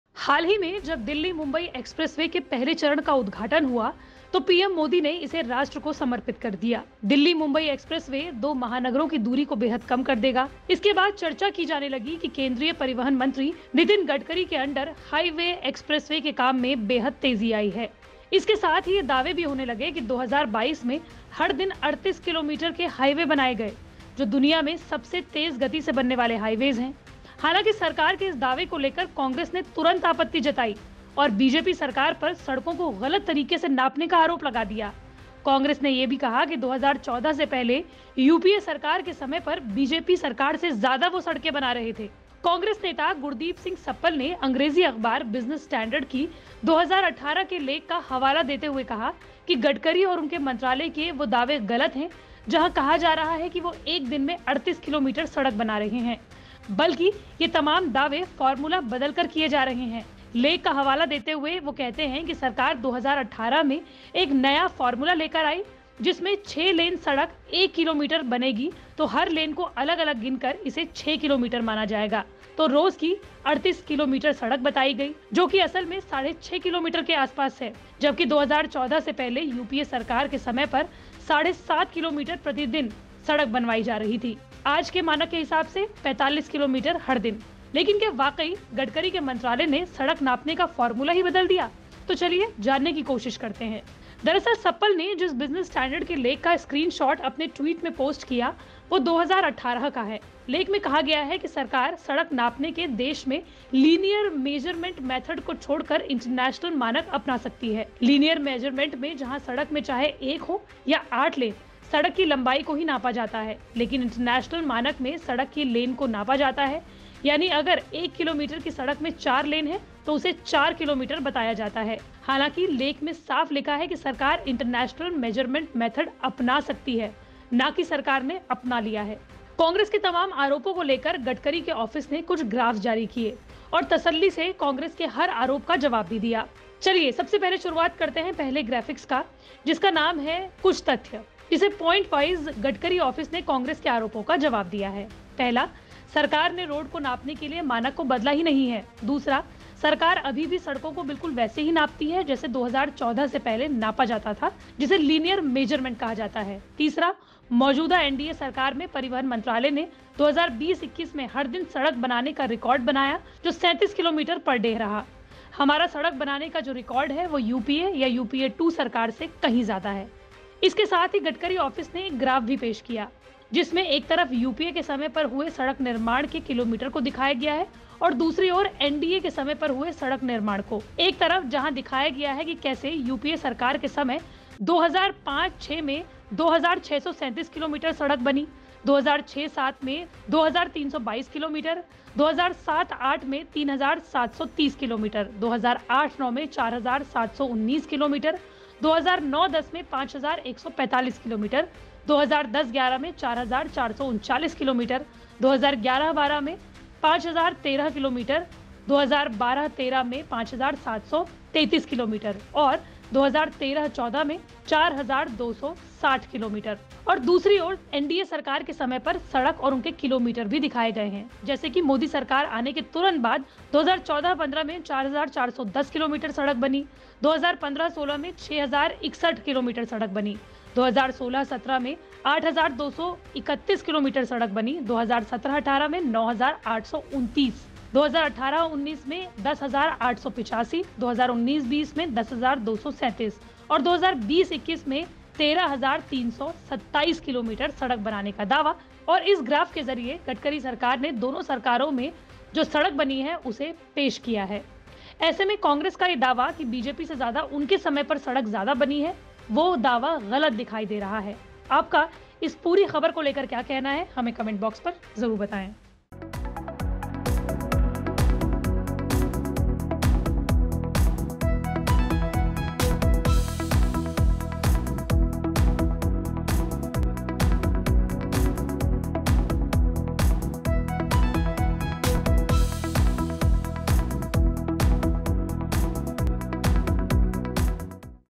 Fact Check: Nitin Gadkari के 38 KM/Day सड़क बनाई, कांग्रेस का दावा यूपीए में बने ज्यादा Highway
हाल ही में जब दिल्ली मुंबई एक्सप्रेस वे के पहले चरण का उदघाटन हुआ तो पीएम मोदी ने इसे राष्ट्र को समर्पित कर दिया..दिल्ली-मुंबई एक्सप्रेस-वे करीब 11088 किलोमीटर का होगा जिससे दो महानगरों की दूरी बेहद कम हो जाएगी.. इसके बाद चर्चा की जाने लगी कि केंद्रीय परिवहन मंत्री नीतिन गडकरी के अंडर हाईवे, एक्सप्रेसवे के काम में बेहद तेजी है.. साथ ही सरकार ने दावा किया है कि 2022 में हर दिन 38 किलोमीटर के हाईवे बनाए गए.. जो दुनिया में सबसे तेज गति से बनने वाले हाई वे हैं.. हालांकि सरकार के इस दावे को लेकर कांग्रेस ने तुरंत आपत्ति जताई.. और बीजेपी सरकार पर सड़कों को गतल तरीके से नापने का आरोप लगा दिया.. कांग्रेस ने ये भी कहा कि 2014 से पहले यूपीए के समय पर बीजेपी सरकार से ज्यादा सड़कें बनाई गईं.. .